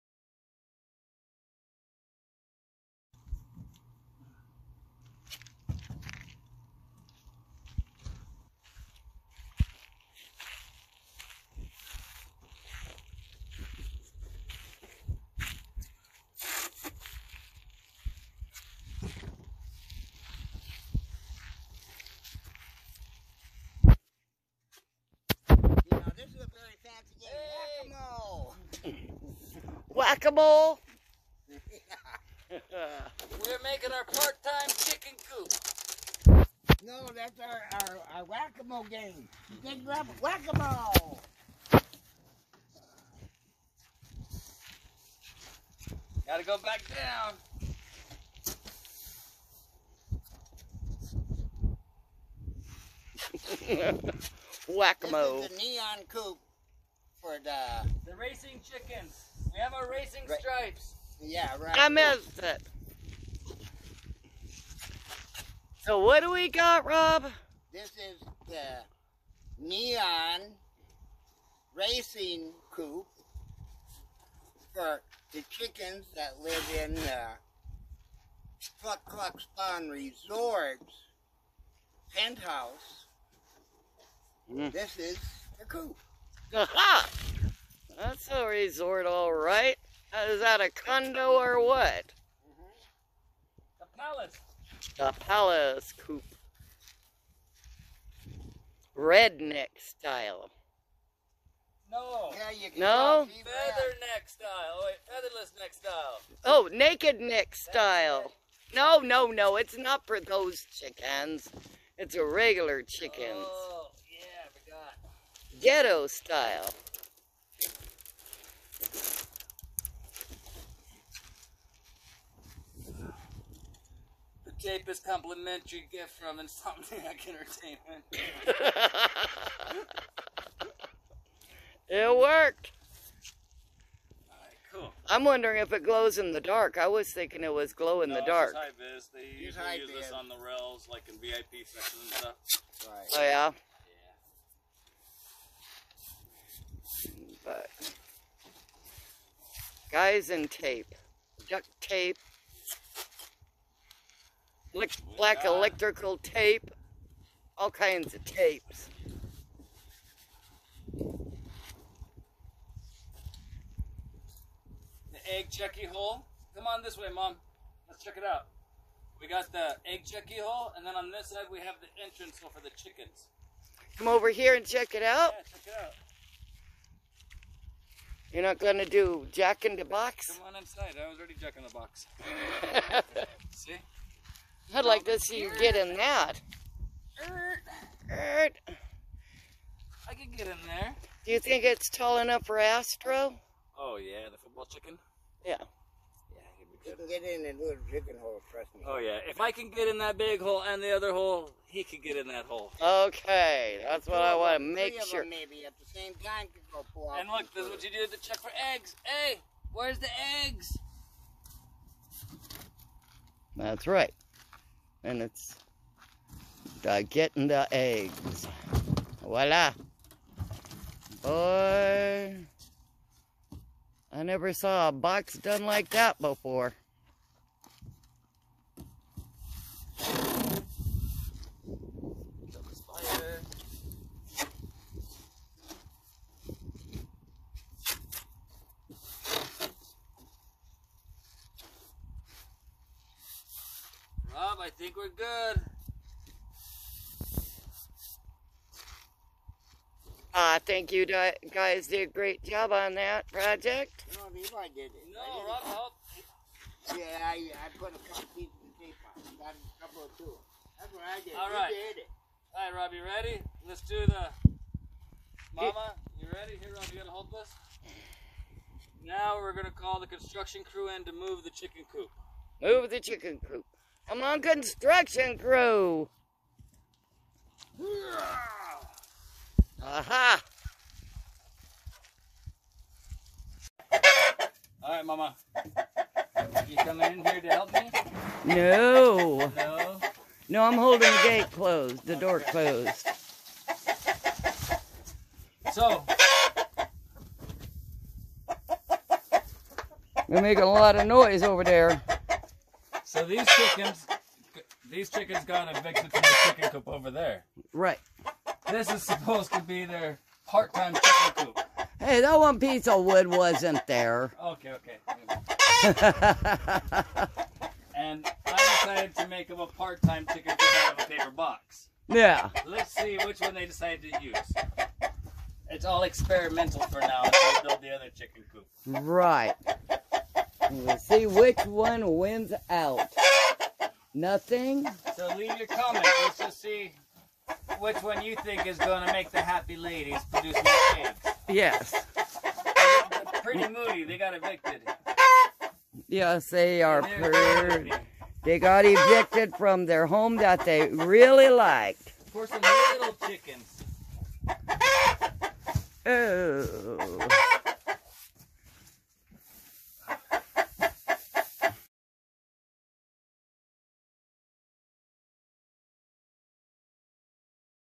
Cheapest Chicken Coop in the Whole
Astro and his girls get a cardboard tiny house.

Plans to build a small coop got postponed when a critter broke into the greenhouse and ate all except two of our tomatoes!

Levi improvised a temporary fix with this cardboard tiny house.